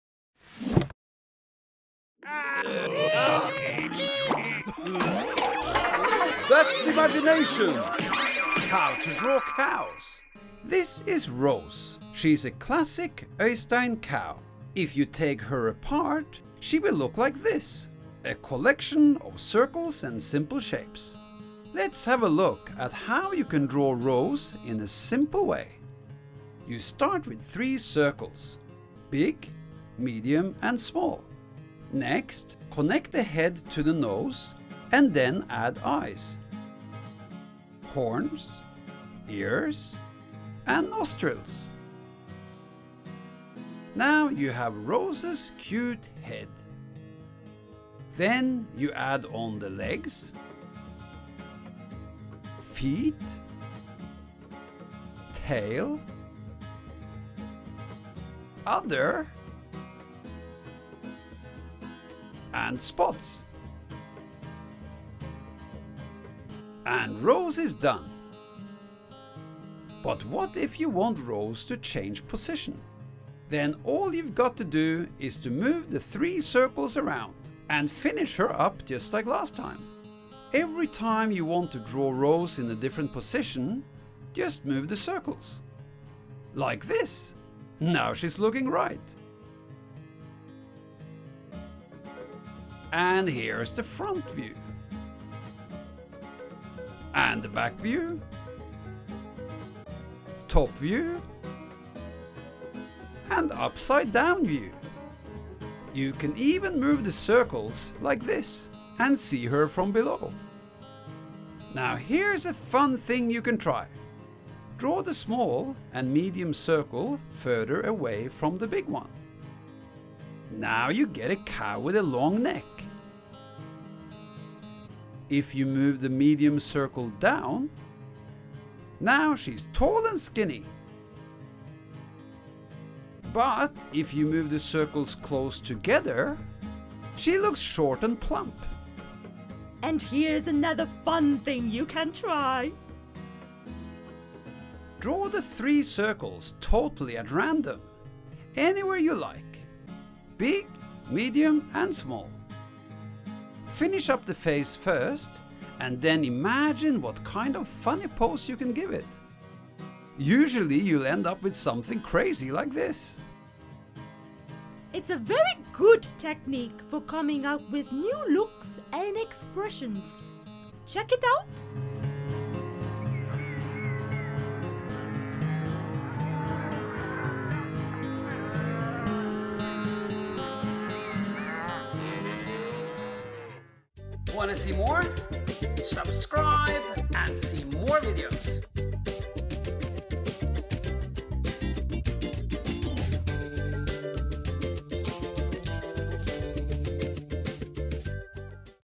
How To Draw A Cow | That’s Imagination | Øistein Kristiansen
Learn how to draw a cow with this easy step by step drawing exercise by Øistein Kristiansen

Every Tuesday and Friday I upload new art videos.